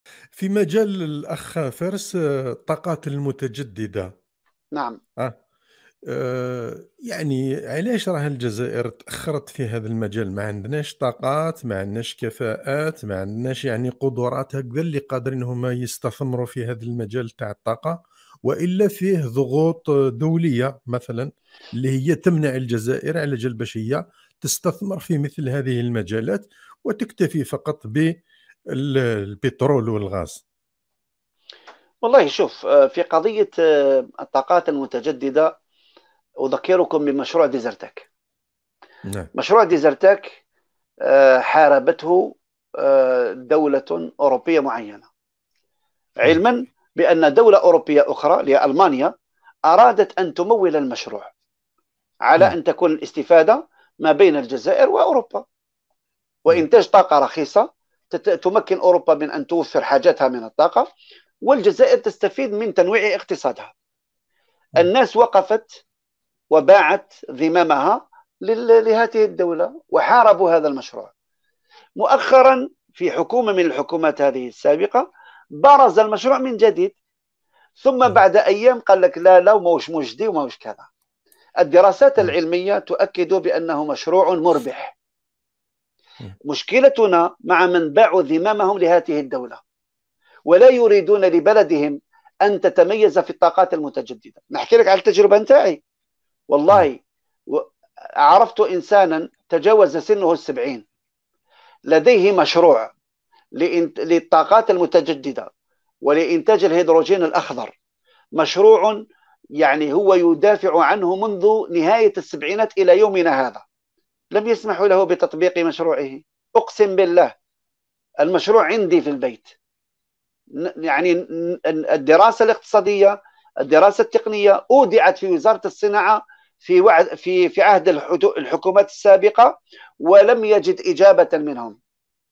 فارس مسدور: لهذه الأسباب تعطل مشروع ديزرتيك، جزائري من السبعينات يعاني لانشاء مشروع في الطاقات
فارس مسدور: لهذه الأسباب تعطل مشروع ديزرتيك، جزائري من السبعينات  يعاني لانشاء مشروع في الطاقات  المتجددة